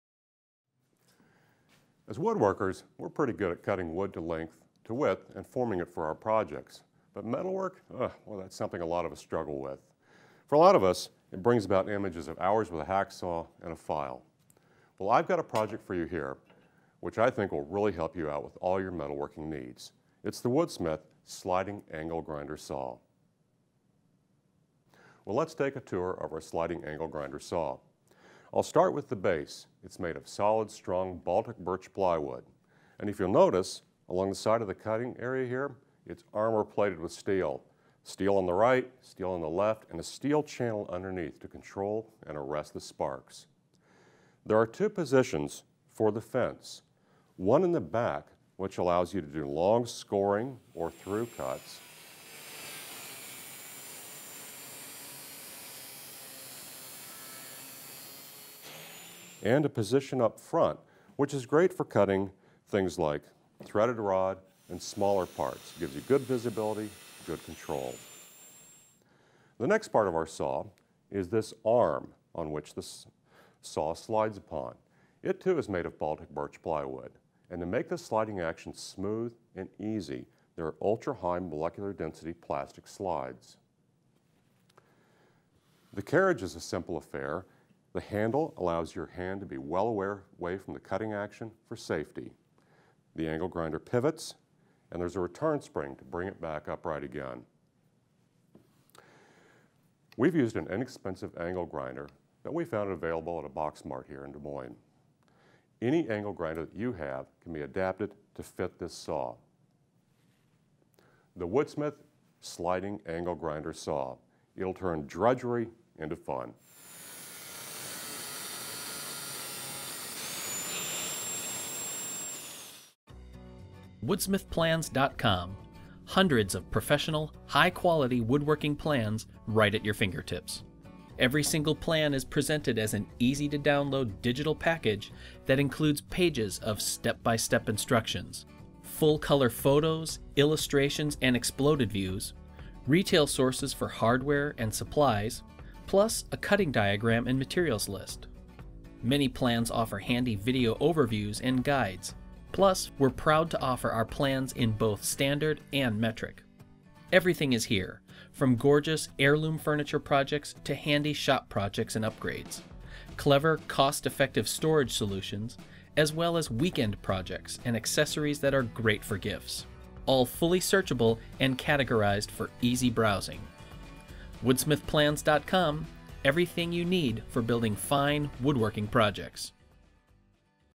Turn Your Angle Grinder Into a Shop-Built Chop Saw!
Get the step-by-step plans to build your own Sliding Cutoff Grinder

Take the mundane task of cutting metal with a hacksaw and turn it in to a fun, easy process by converting your angle grinder into a sliding chop saw with some plywood and a few common pieces of hardware.

What makes this project work are the clever carriage and handle assembles. These hold the grinder and allow it to slide back and forth, as well as rotate up and down. The assemblies slide along a hardwood rail, which in turn is connected to a sturdy support arm that’s bolted to a rock-solid base. It also has a wide metal channel at the center for added safety, and a two-position fence for greater versatility. All in all, it’s sure to add some precision to your metal-cutting operations.